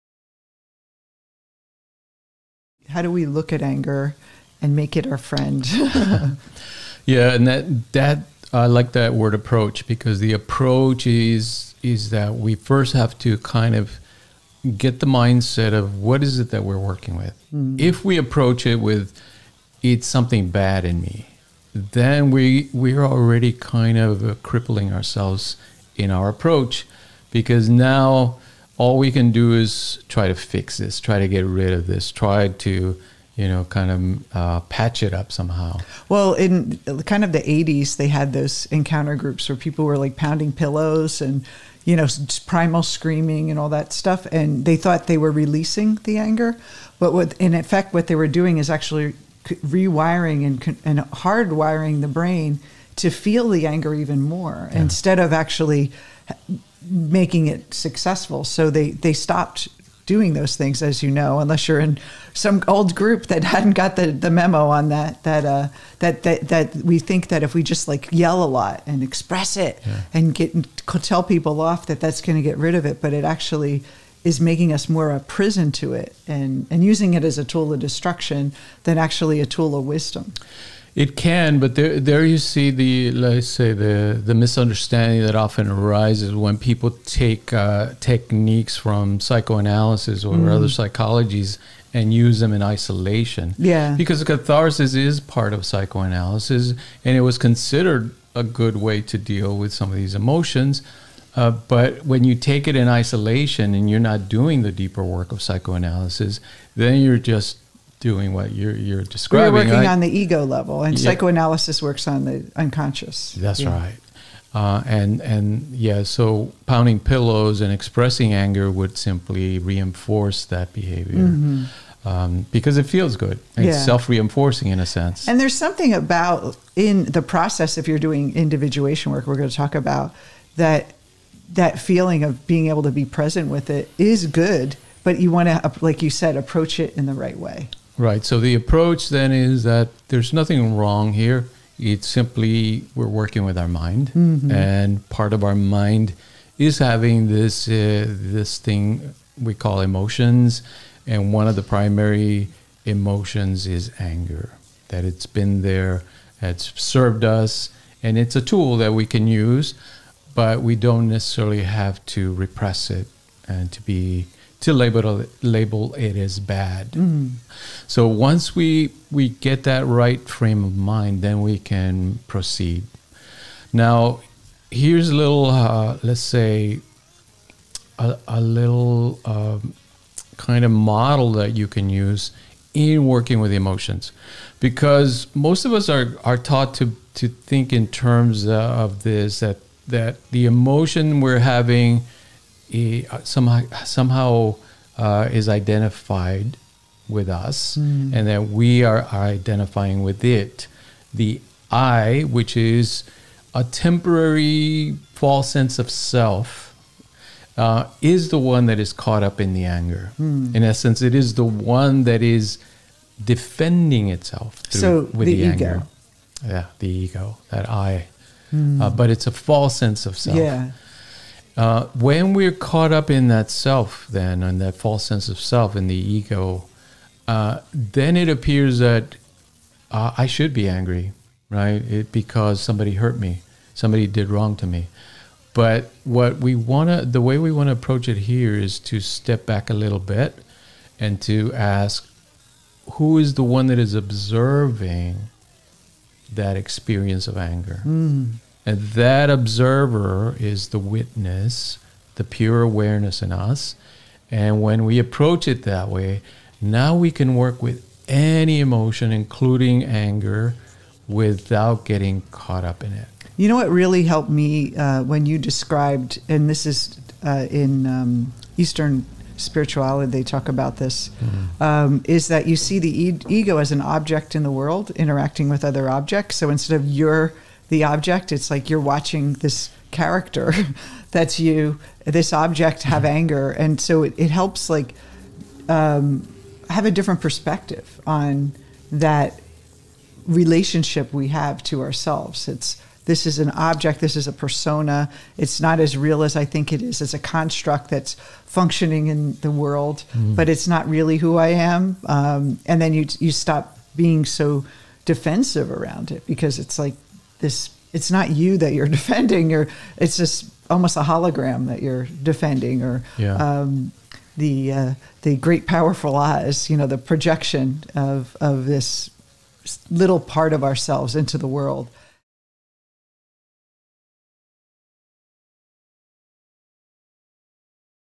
How to Make Anger a Friend | Jungian Life Coaching
The approach to working with anger should not be to push it away as something bad but to understand that it is a tool we can use to understand ourselves better. Often we get caught up in anger by over-identifying with the emotion because we feel that we have been wronged, or we try to push it away because it is not socially acceptable. In either circumstance, we give our power over to the emotion. We need to step back and observe the anger from the witness mind.

for courses and content.